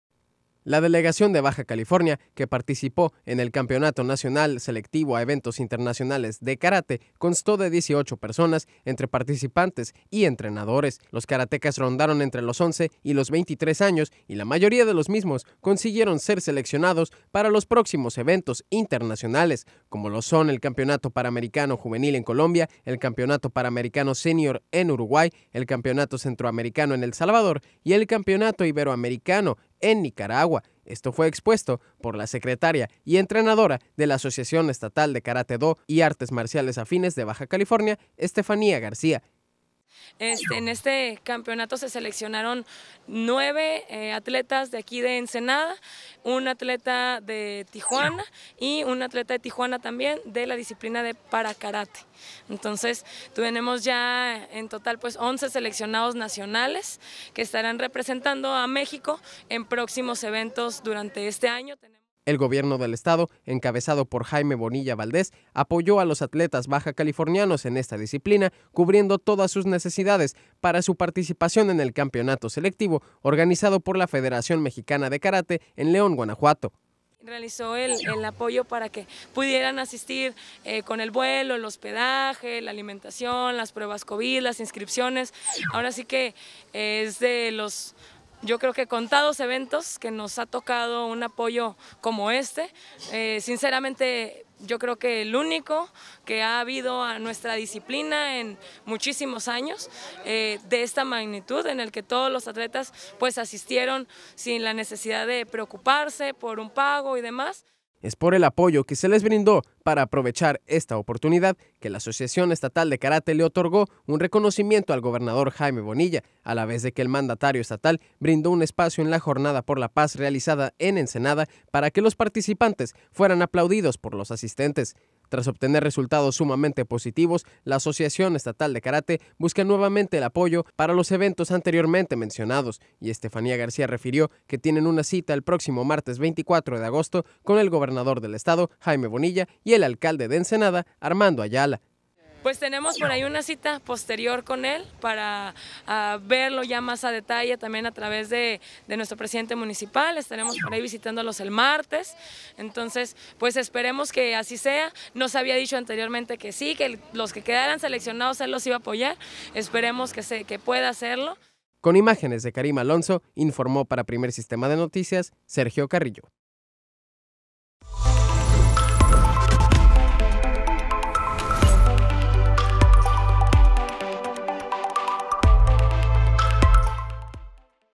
Karate Selectivo Nacional
La delegación de Baja California que participo en el campeonato nacional selectivo a eventos internacionales de Karate, consiguen ser seleccionados para próximos eventos internacionales.

Campeonato Panamericano Juvenil en Colombia
Campeonato Panamericano Senior en Uruguay
Campeonato Centroamericano en El Salvador
Campeonato Iberoamericano en Nicaragua

Estefanía García  es la secretaria y entrenadora de la Asociación Estatal de Karate Do y Artes Marciales Afines de Baja California A.C.